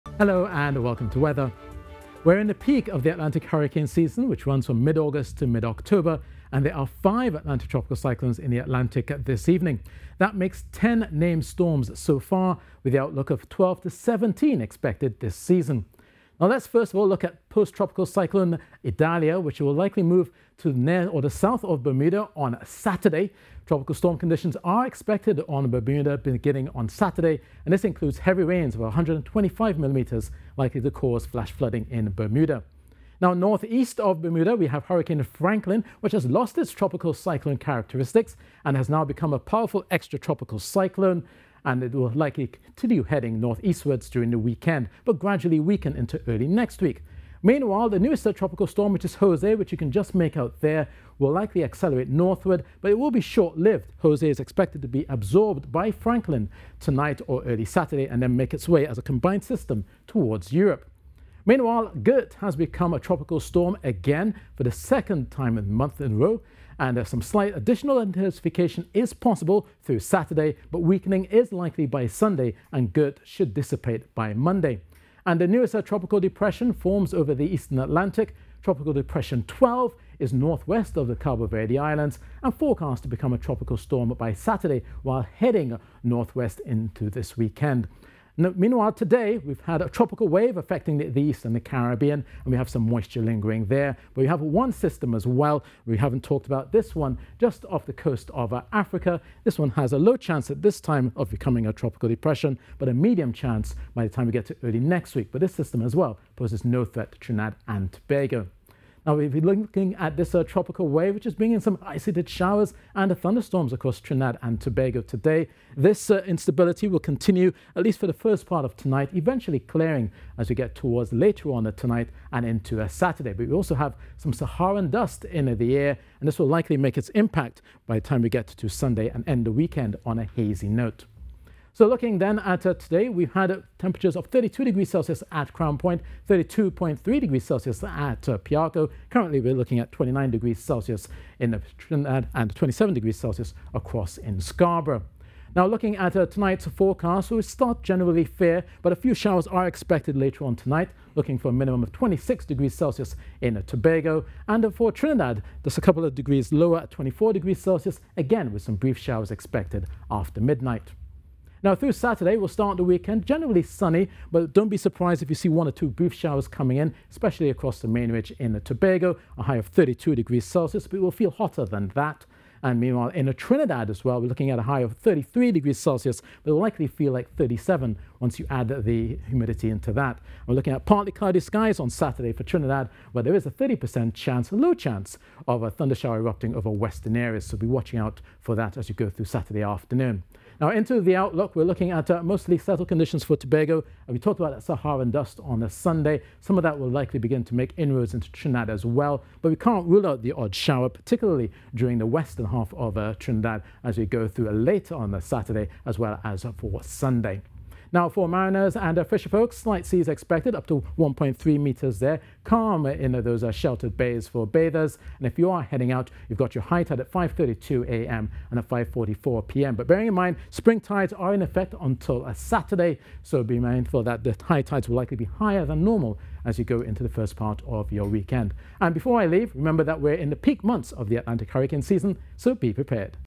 Weather Outlook - Friday September 1st 2023
There are five Atlantic Tropical Cyclones in the Atlantic Friday evening.

That makes 10 named Storms so far, with the outlook of 12 to 17 expected this season.